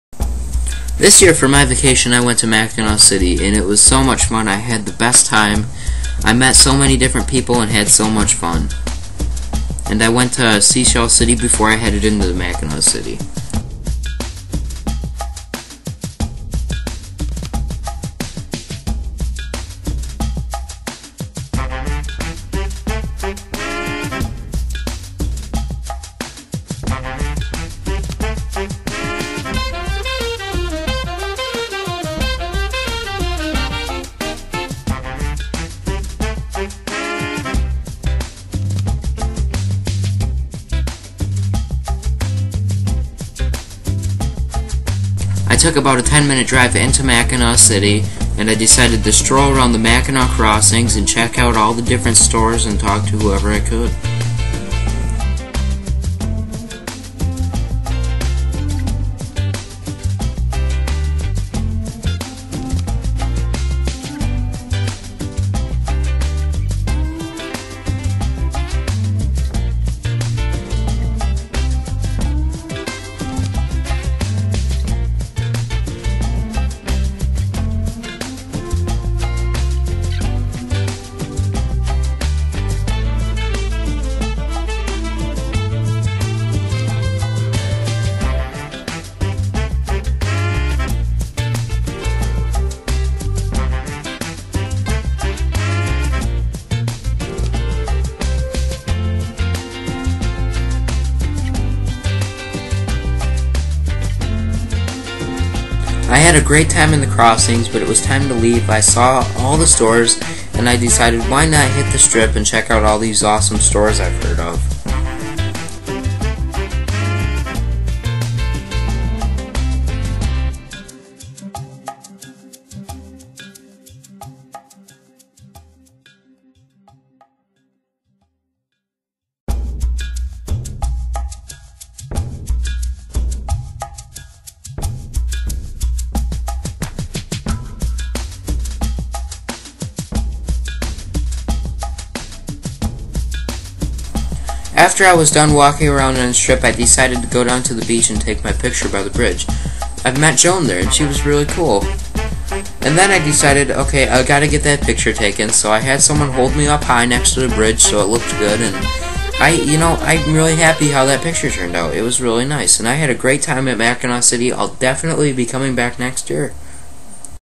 Suzuki Sam Goes On A Vacation
Suzuki Sam Goes On A Vacation to Mackinaw City, Michigan.